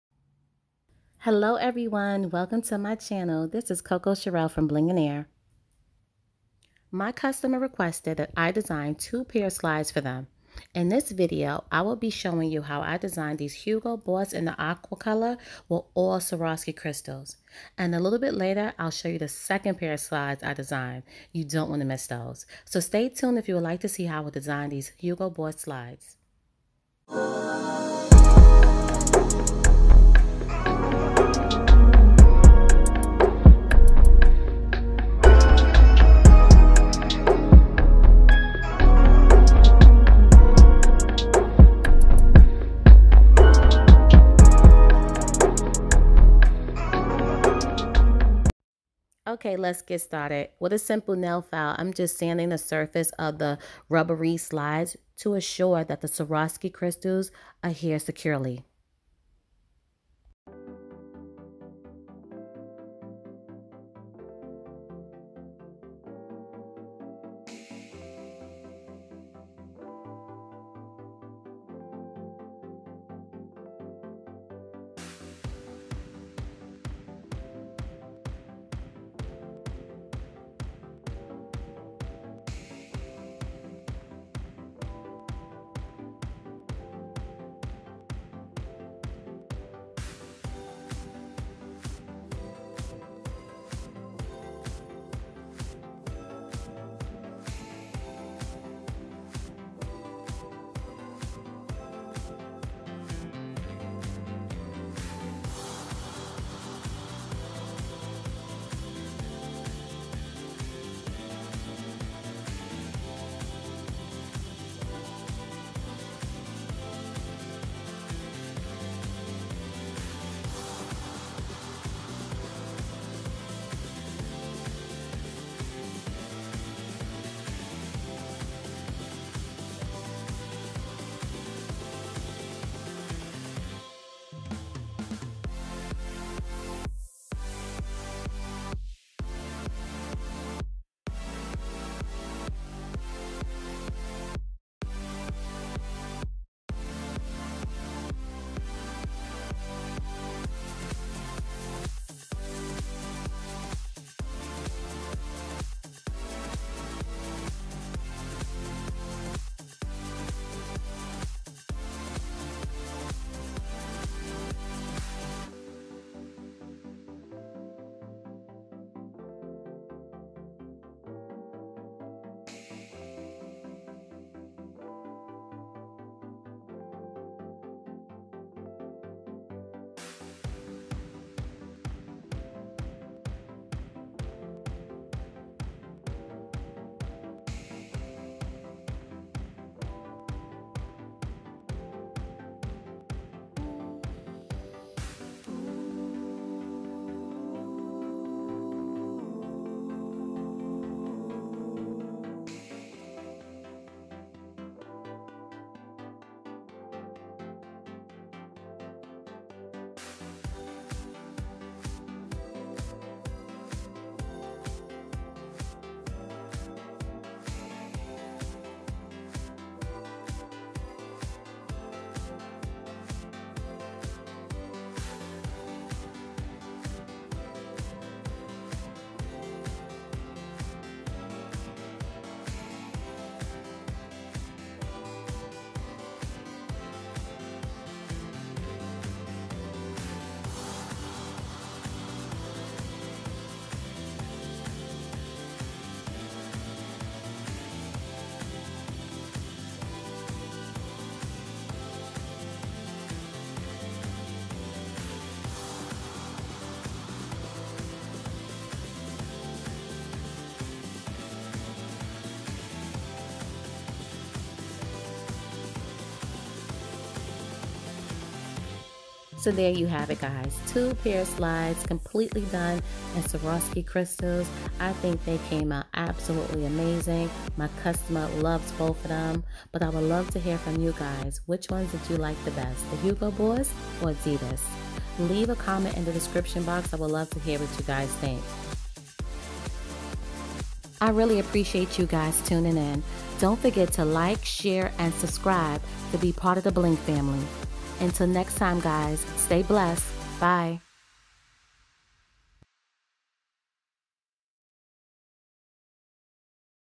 DIY Hugo Boss Swarovski Crystals Vacation Slides
Hello Bling family thank you for tuning in. I have designed two pair of slides for my customer. They both came out amazing!

HUGO BOSS:
Swaroski Crystal color: Chrysolite
Crystal Sizes: ss10, ss20

ADIDAS:
Swarovski Crystals color:
Crystal Aurora Borealis
Jet Hematite
Crystal Sizes: ss10, 12, 16, 20

E6000 Glue, White Wax Pencil, Dotting Tool.